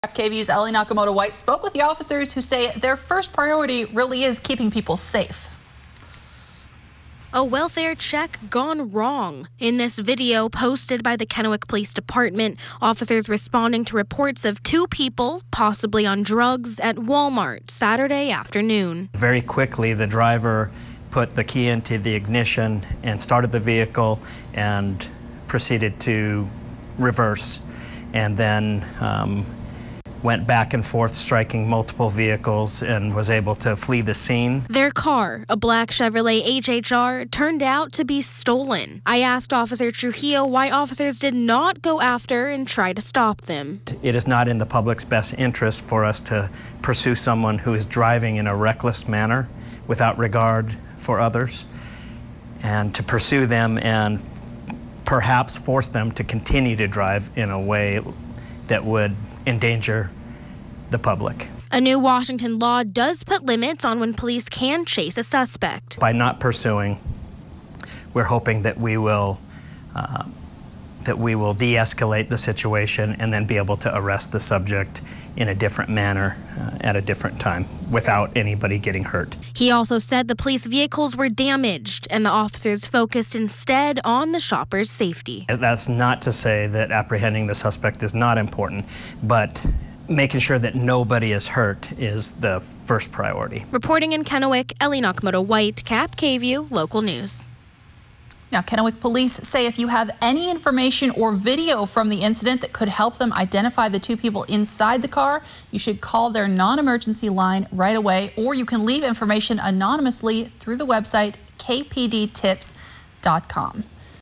KPD officers still searching for suspects who damaged multiple cars while evading arrest at Walmart
Ellie Nakamoto-White reports.